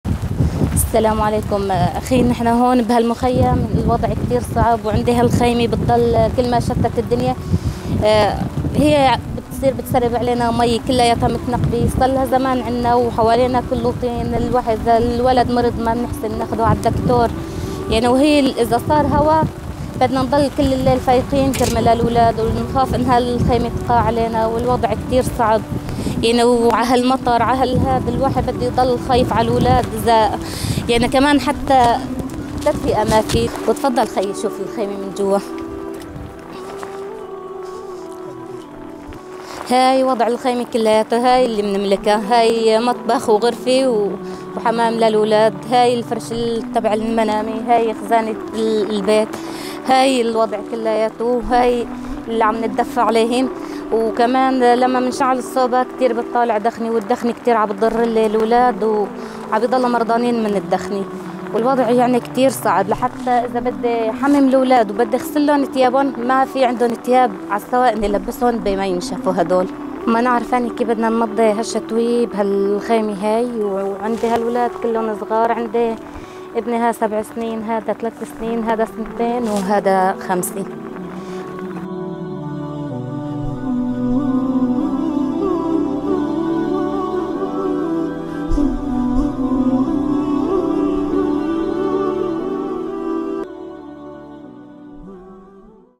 A widow in Syria struggles every winter
Please watch how the winter conditions affect displaced families and widows in Syria where temperatures are below freezing.